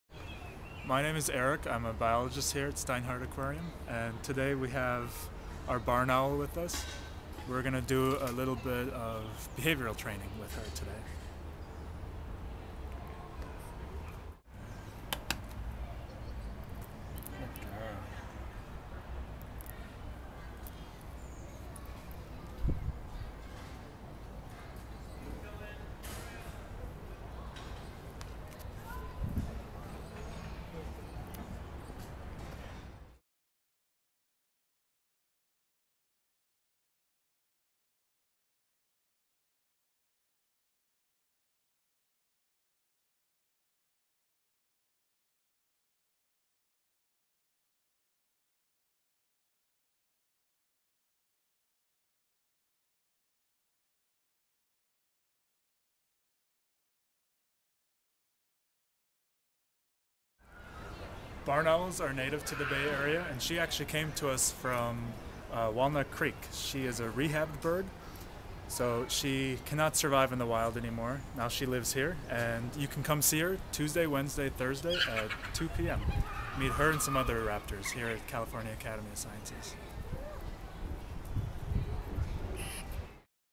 Behind the Scenes: Barn Owl Training | California Academy of Sciences
Academy biologist Eric provides a behind the scenes look at training the Academy's Barn Owl (Tyto alba). Barn Owls are native to the San Francisco Bay Area and are the most widely distributed species of owl. Their body length can reach 40 centimeters (15.5 inches) with a 110 centimeters (43 inch) wingspan. These nocturnal birds have excellent low-light vision but they find prey using asymmetrically placed ears that localize sounds generated by voles, pocket gophers, mice and rats.